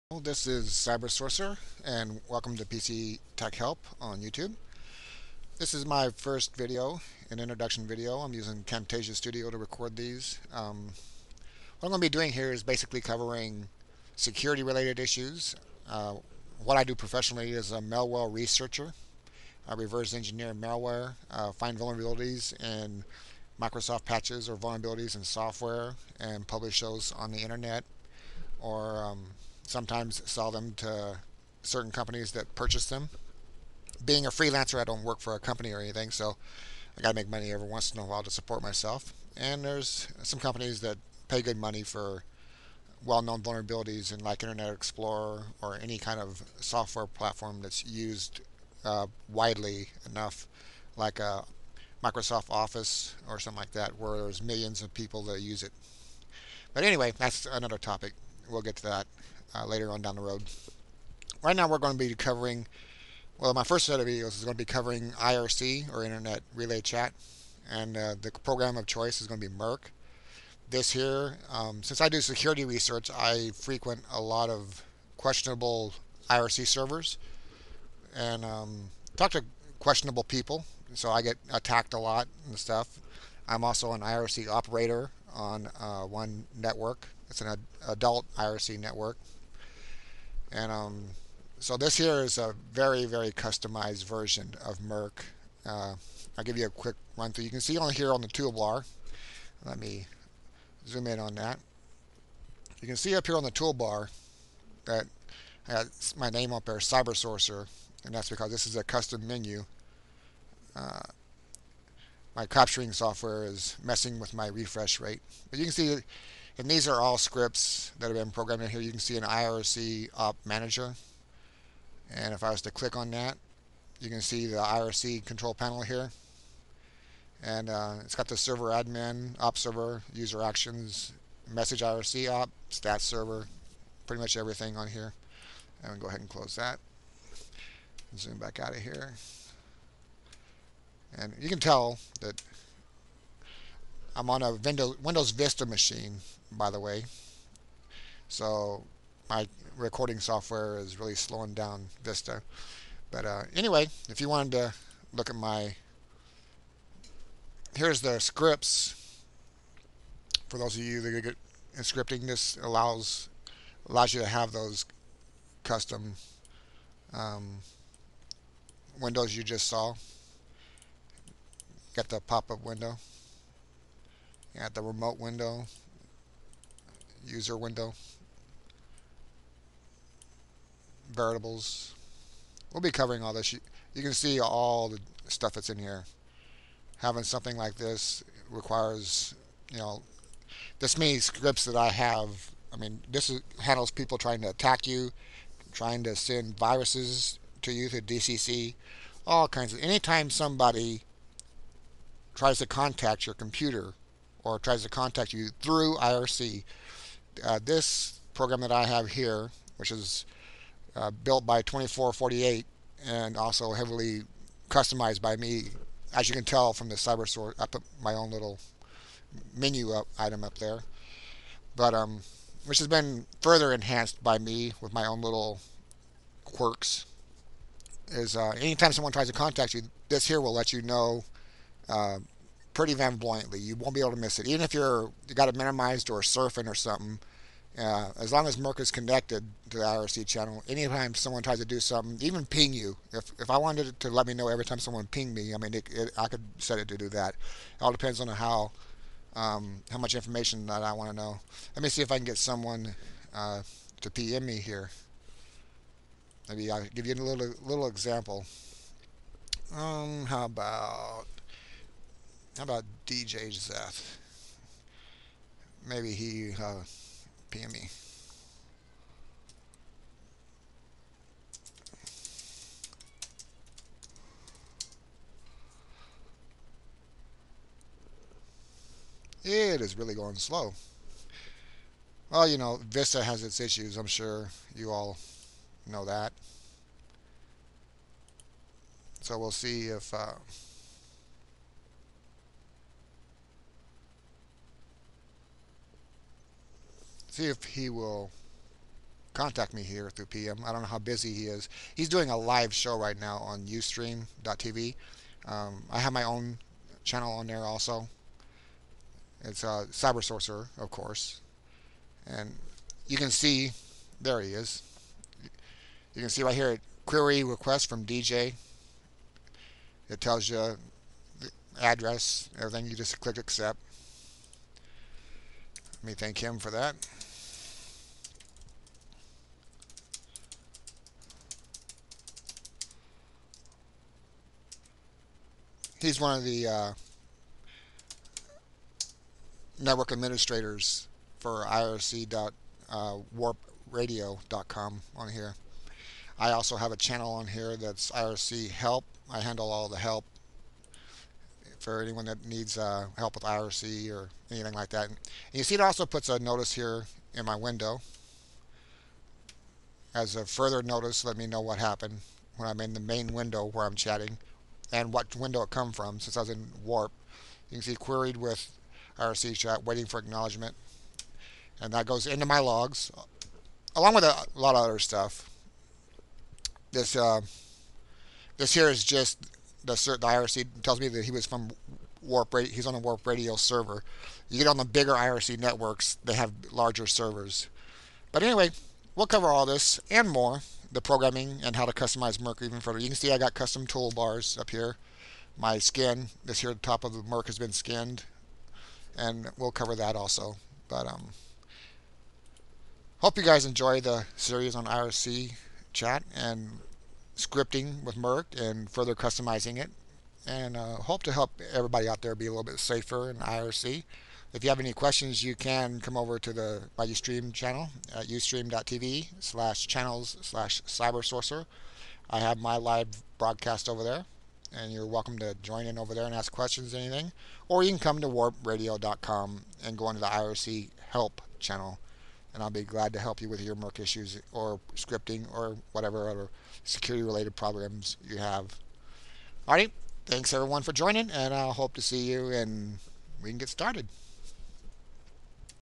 Mirc Intro
Basic intro to the Mirc IRC chat client that will be coming. Its main focus will be on scripting.

IRC Chat = irc.dal.net pctech-help